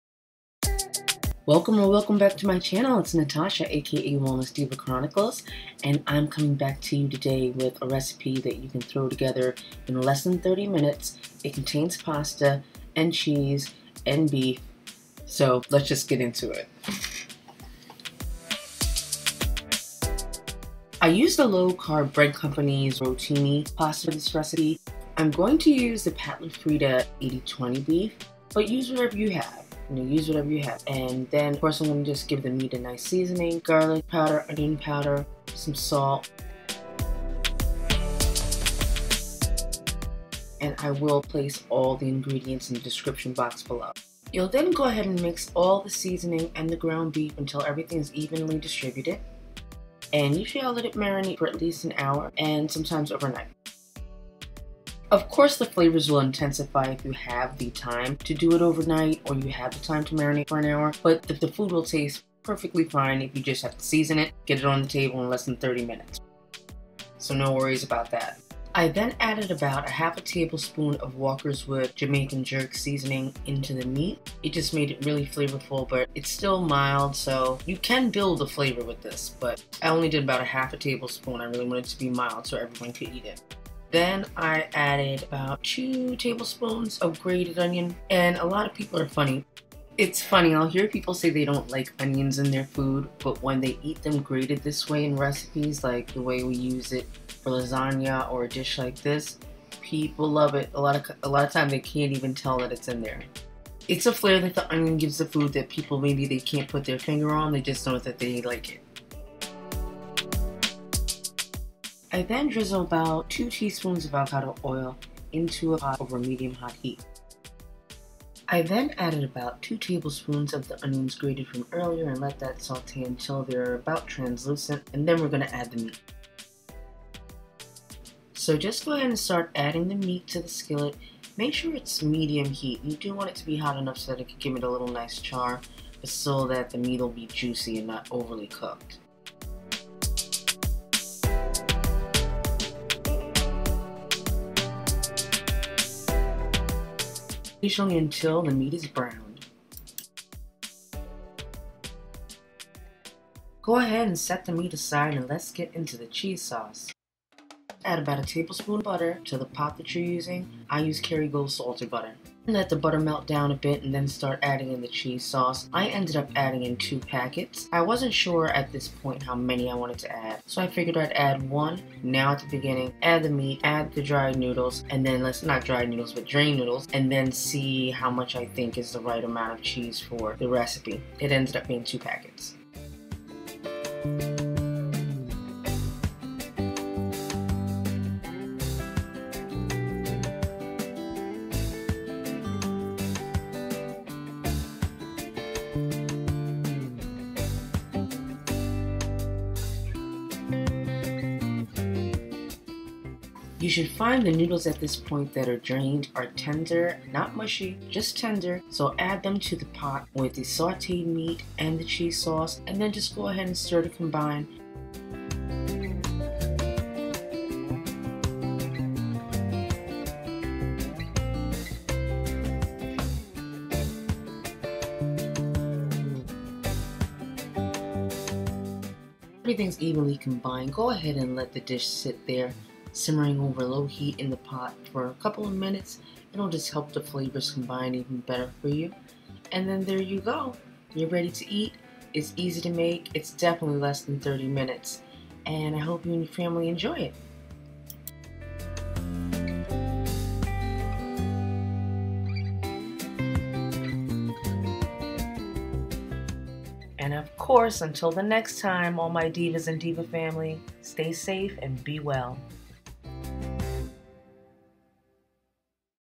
HAMBURGER HELPER (DUPE) PASTA
Most of us likely remember some sort of Hamburger Helper as a part of our childhood. But people are under the impression that satiating and excellent texture is non-existent in the realm of ketogenic options.

Fortunately, I have found some pre-made options and am interested in attempting some keto pasta from scratch-may be a nice project to make with my son).

This recipe is quick and easy and uses many ingredients that we can still access in bulk (not people fighting like they have no sense bulk, but you get me).

You'll only need a few ingredients to make this super-simple, satisfying, and creamy pasta dish. And I am going to show you exactly how to make the dish in this straightforward video; great for when you need to get meals on the table, now.

MACROS: About eight 1 + 1/4 cup servings | Cals: 240 | Fat: 17g | Total Carb: 6g (4g net carb per seuggested serving) | Fiber: 2g | Protein: 14g

Ingredients:

1 lb (16 oz.), 80/20 ground beef
2 packets, Velveeta cheese sauce
1 cup, THe Low Carb Bread Company (Rotini)
2 tbsp, white onion (grated)
2/3 tbsp, onion powder
1/2 tbsp, garlic powder
1/2 tsp, Kosher salt
1 tbsp, Kerrygold (or whatever you have) butter
2 tsp, Avocado oil, or whatever you have on hand to sautée the onions before searing the meat.

Products Used:
The Low Carb Bread Co. Rotini Noodles
Velveeta Cheese Sauce, Original, 3 - 4oz Pouches (Pack of 4)